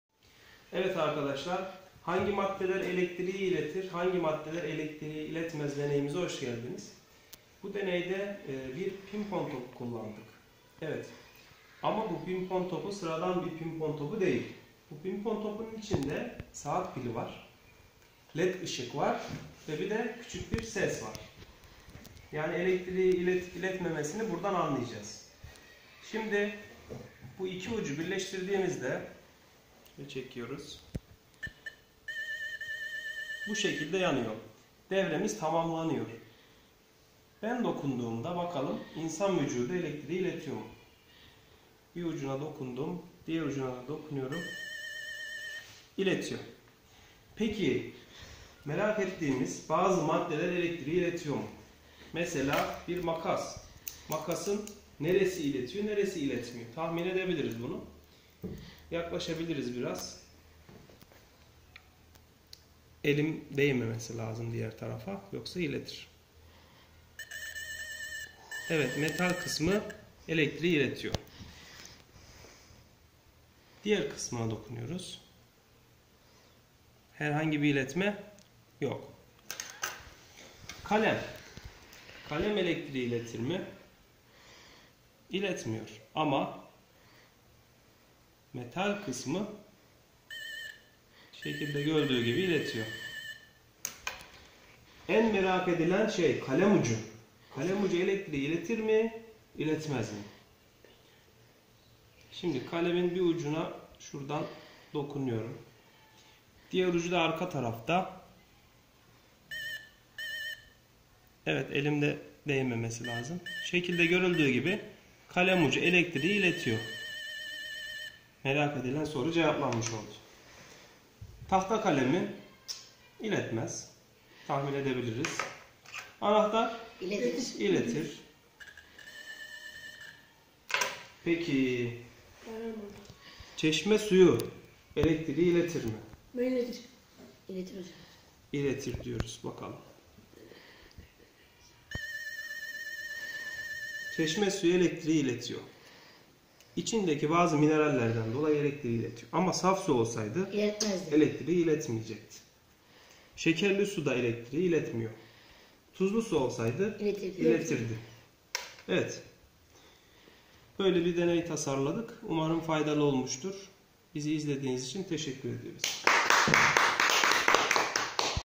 Çok ilginç bir deney, Pinpon topu ile elektrik deneyi, Enerji topu
Pinpon topu mucizesi.. Pinpon topunun içine yerleştirilen alarm ve ışık sayesinde hangi maddelerin elektriği ilettiği hangi maddelerin elektriği iletmediğini bu deneyde öğrencilerinize kavratabilirsiniz.